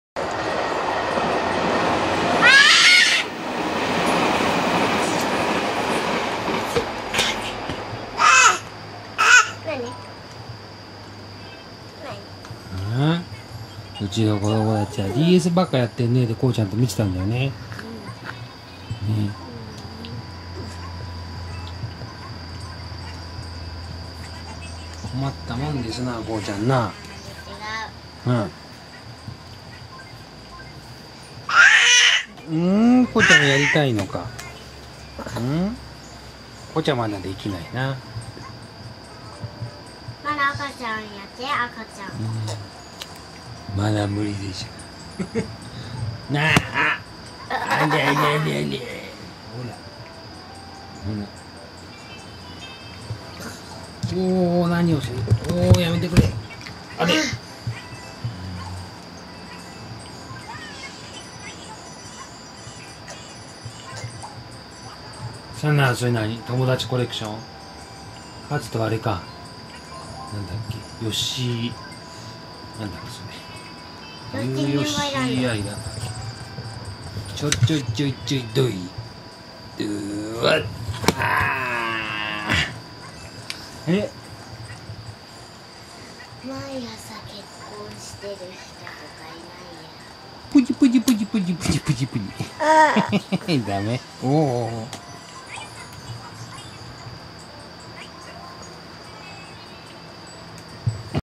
ゲーマー兄弟
今年の誕生日プレゼントは２人共3DSのゲーム。
２人でピコピコゲーム熱中。遊んでもらえないこうちゃんは不満そうです。